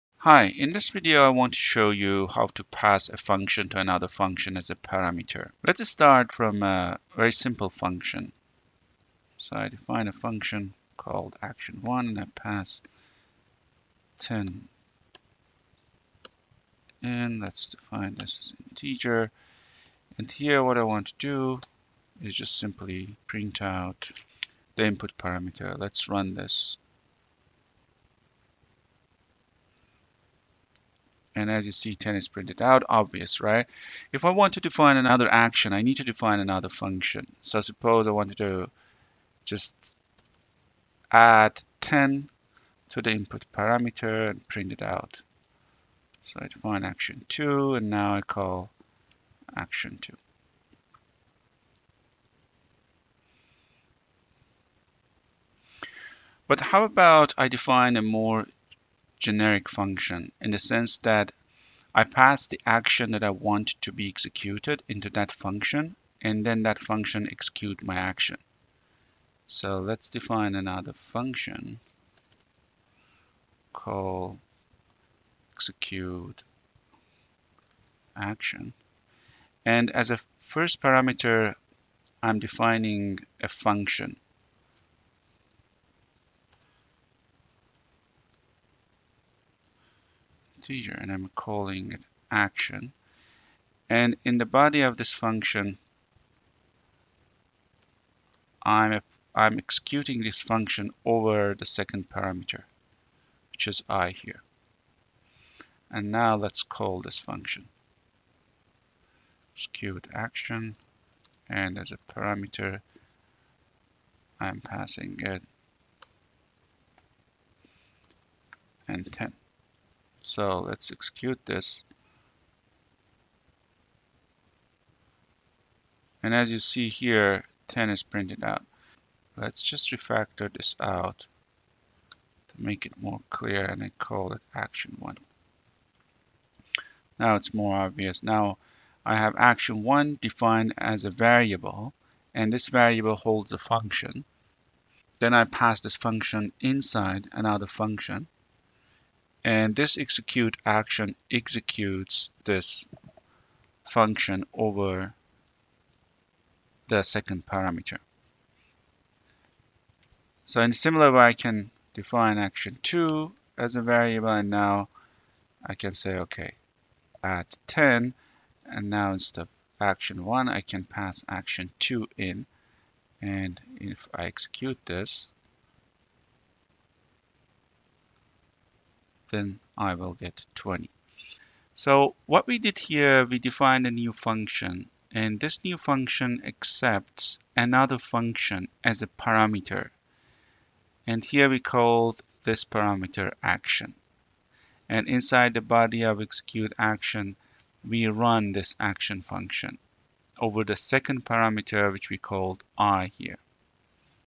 How to Pass a Function as a Parameter to Another Function in Java
In this short video I show how to pass a function to another function as a parameter.